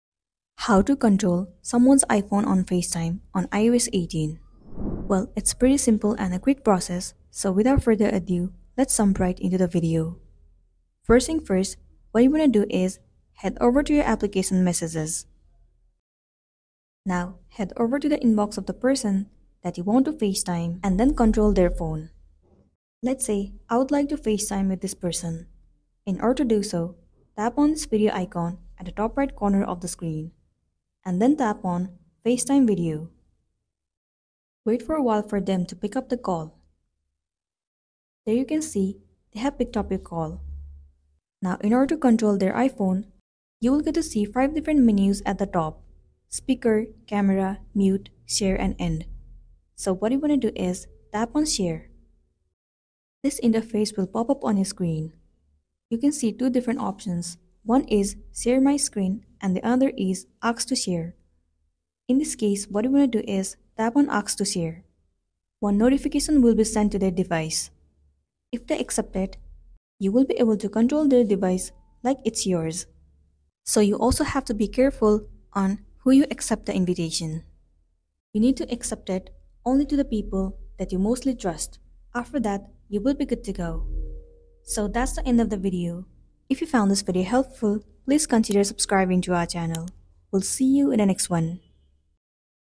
How to Remotely Control Someone's iPhone or iPad (NEW iOS FEATURE)
**How to Remotely Take Control of an iPhone or iPad (NEW iOS FEATURE) 📱💻 | Control Devices in 2024**

Want to control an iPhone or iPad from a distance? This guide will show you how to use the new iOS feature that allows you to remotely take control of an iPhone or iPad, making troubleshooting and device management a breeze.

In this video:
0:00 - Introduction
0:05 - How To Control Someone's iPhone Screen On FaceTime

We’ll explain the new iOS feature that lets you remotely access and control an iPhone or iPad. Learn how to set it up through Apple’s **Remote Management** tools, including the steps to grant access, manage permissions, and troubleshoot devices without being physically near them. This feature is perfect for tech support, device management, or helping a friend or family member with their iOS device.

Master this new iOS tool to effortlessly manage iPhones and iPads remotely in 2024!